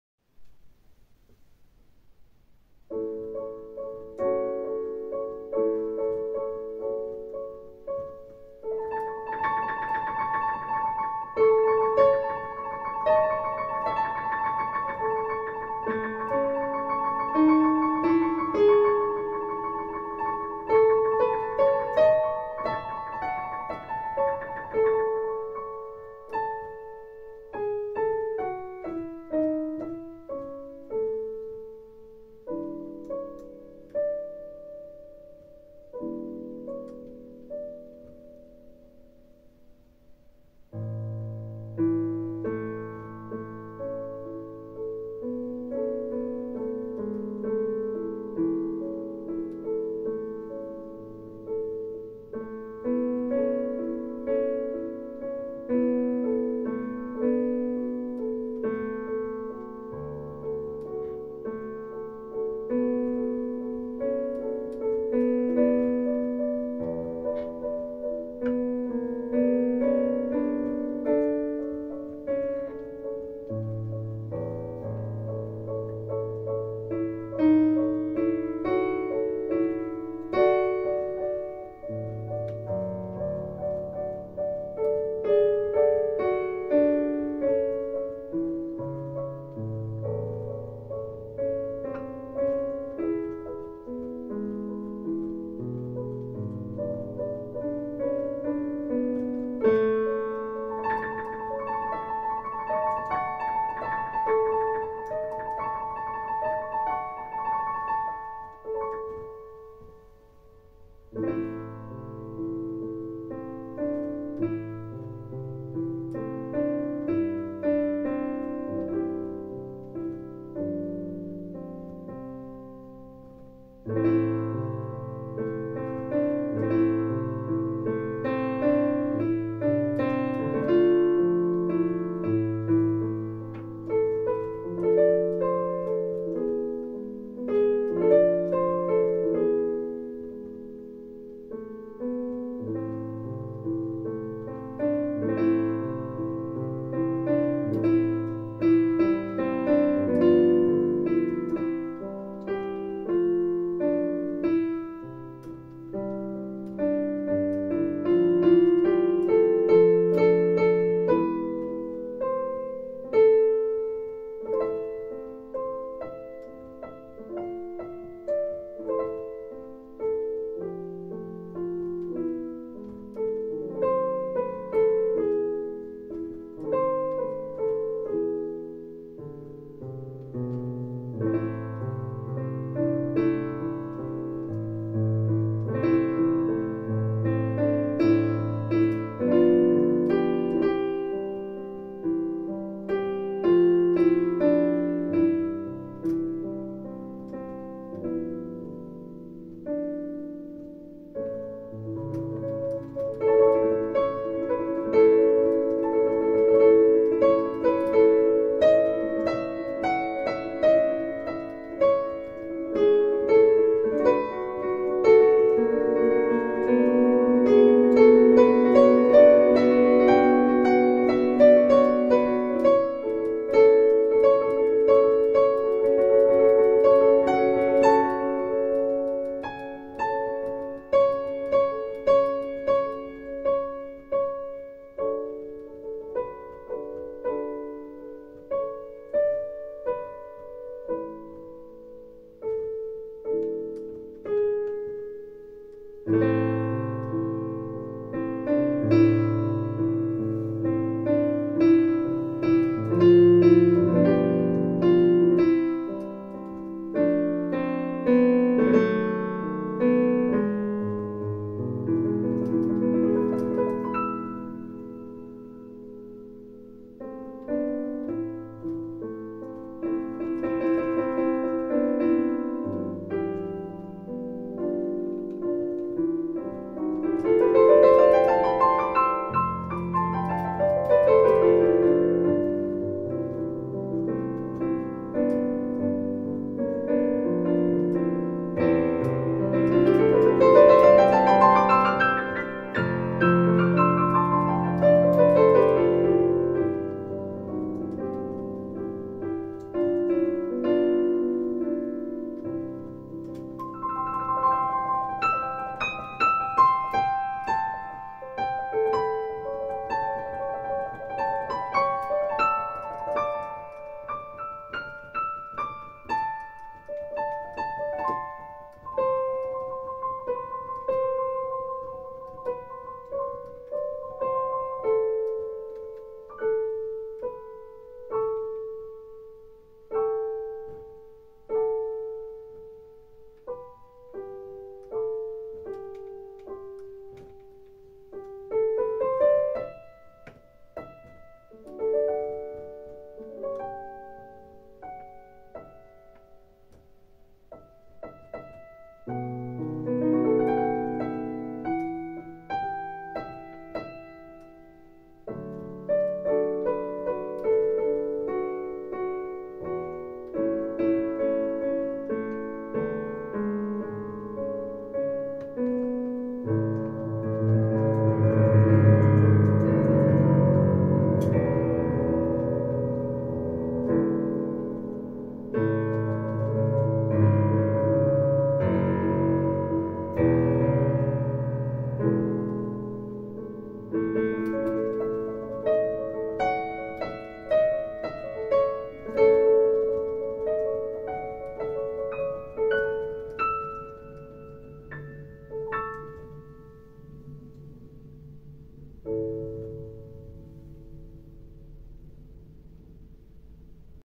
Musical Museum: B. Morisot - Eugène Manet on the Isle of Wight (1875)
Piano improvisation by Alessio Barcellini ( 2024 )